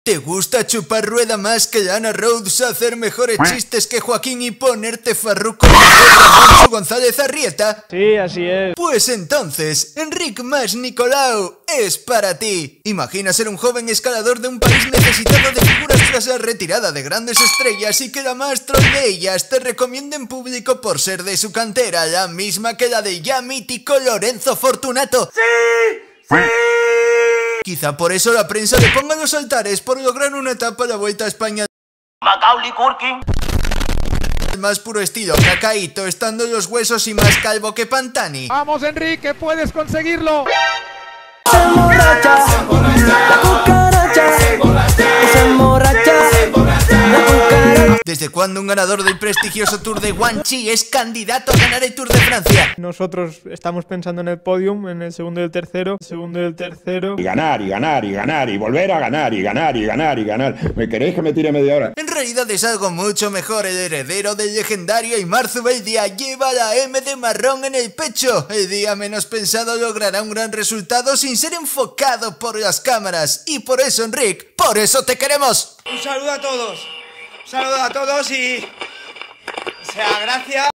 ENRIC MAS EN 1 MINUTO 🇪🇸
Enric Mas no ganará el Tour de Francia 2021 ni la Vuelta a España 2021 si no realiza ningún ataque pero desde Movistar Team Abarca estarán muy contentos con su Maillot Marrón de Haimar Zubeldia conquistado con todos los honores, ya lo dice Perico Delgado; "si es que hay que ponerse en la piel de los equipos, Carlos, valoran mucho un puesto entre los 10 primeros"; en eso nuestro amigo Enric Mas Nicolau es todo un crack, si no que le pregunten a su mentor Alberto Contador